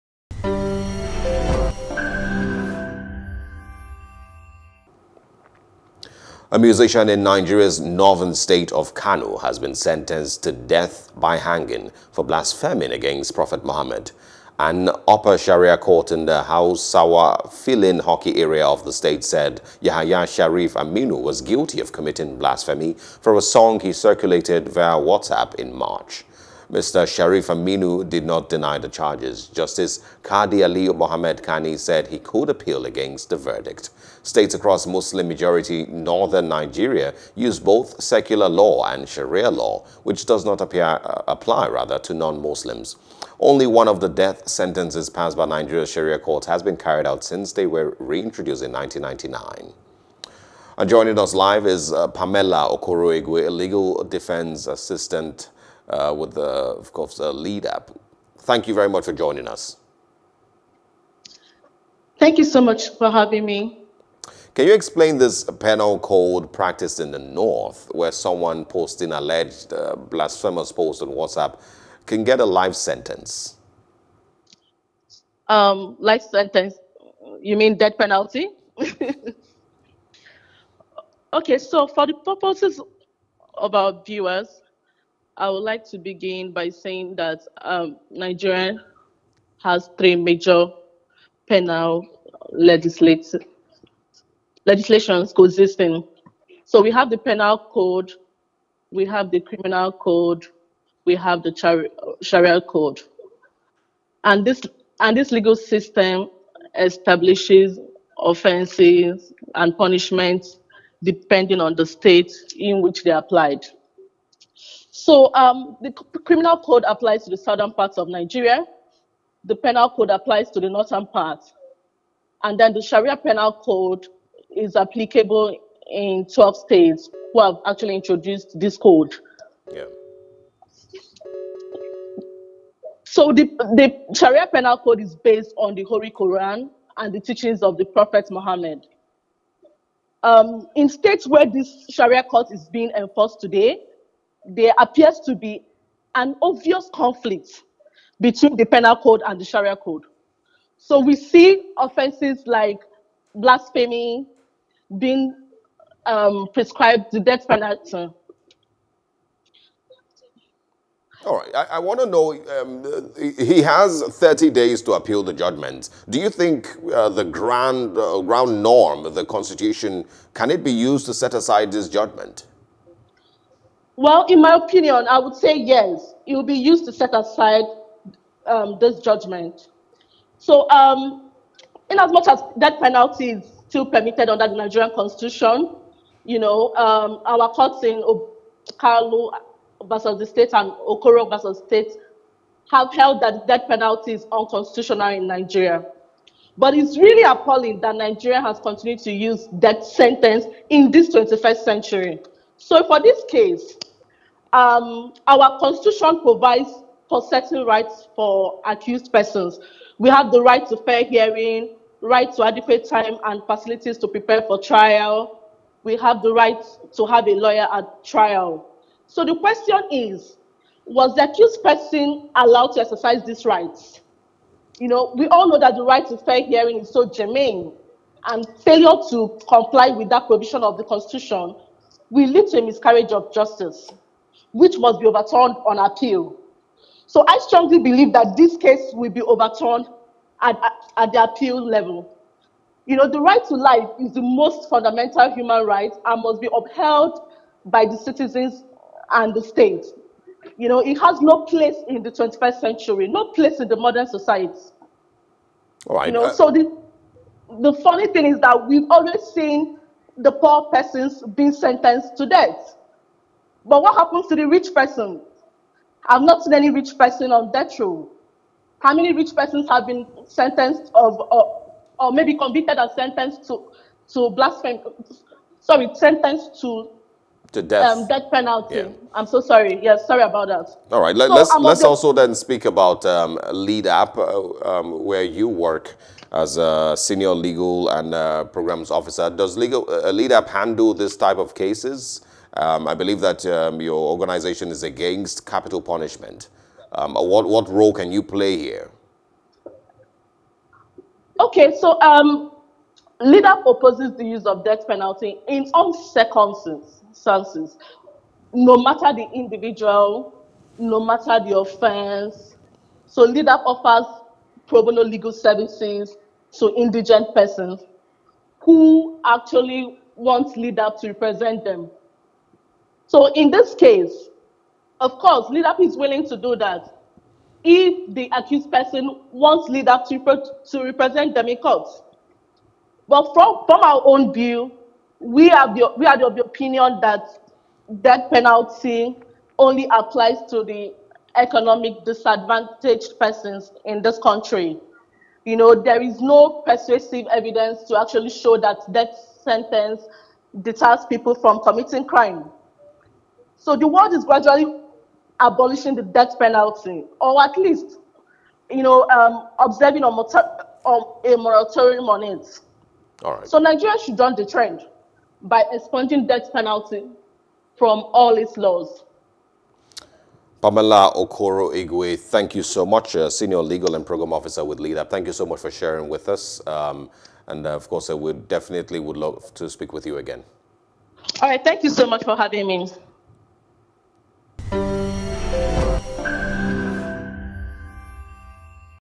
Yahaya Sharif-Aminu - A Nigerian Singer Sentenced to Death for Blasphemy Against Prophet Muhammad
A musician in Nigeria's northern state of Kano has been sentenced to death by hanging for blaspheming against the Prophet Muhammad. An upper Sharia court in the Hausawa Filin Hockey area of the state said Yahaya Sharif-Aminu, 22, was guilty of committing blasphemy for a song he circulated via WhatsApp in March.